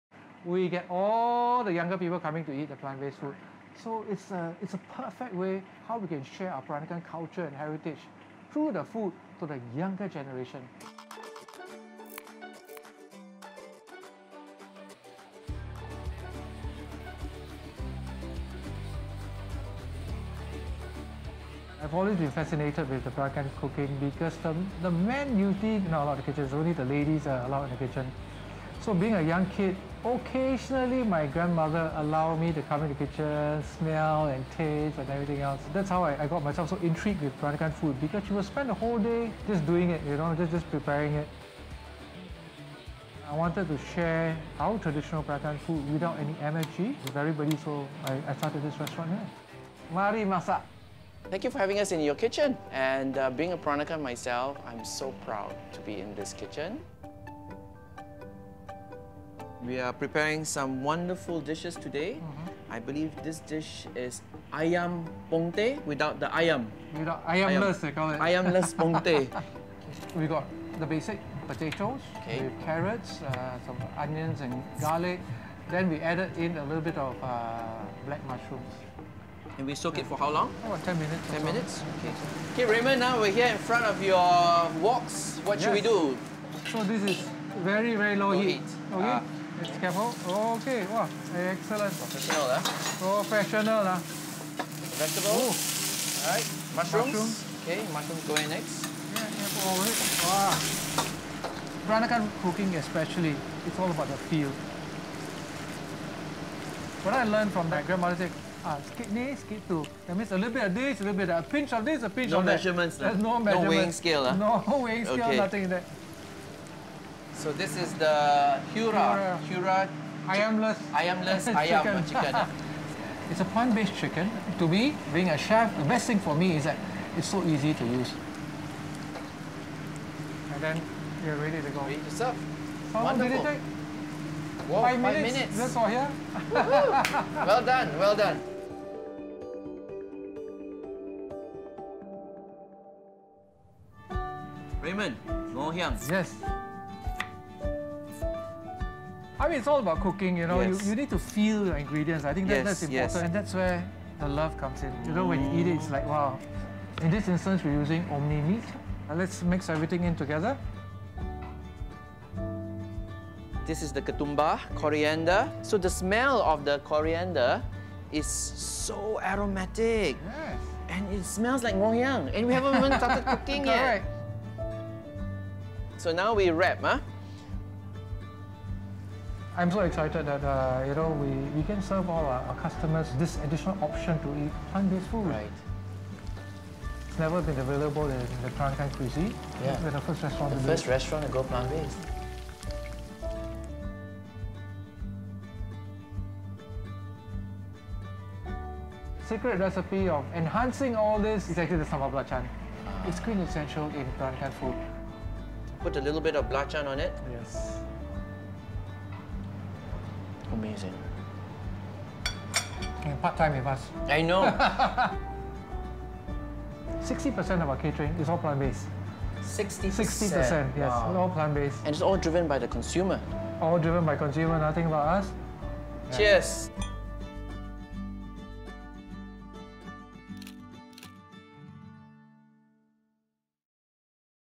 Singapore goes plant-based: Can Food Innovations Revive Traditions?
How do traditional Peranakan flavours stay fresh and relevant today? We talk to executive chef-owner of 'The Peranakan', Raymond Khoo, and learn how he reinvents old recipes using plant-based foods like OmniMeat and Heura to birth new creations. Will this do the trick to appeal to the millenials?

Feel Free to Know More About Us!